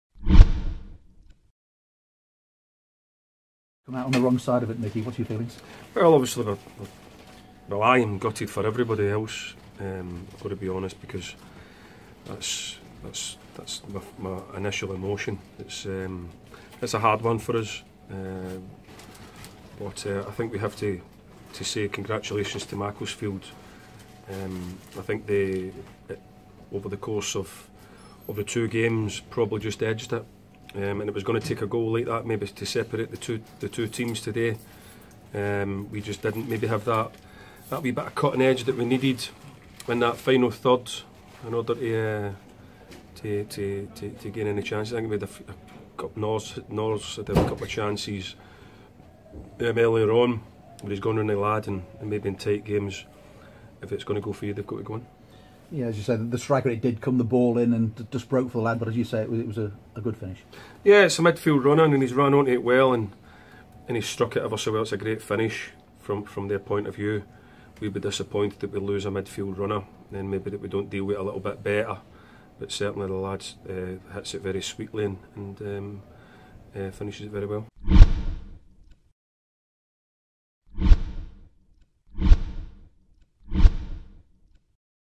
POST-MATCH: Micky Mellon (Macclesfield Town H)
Micky Mellon could not hide his disappointment at defeat in the semi-final of the Buildbase FA Trophy against Macclesfield Town.